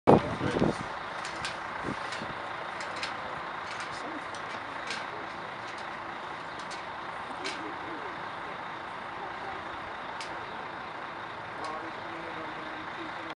Wind Sculpture Port Isaac 2013
Wind Sculpture by martiensbekker.co.uk in Port Isaac (Portwenn) Cornwall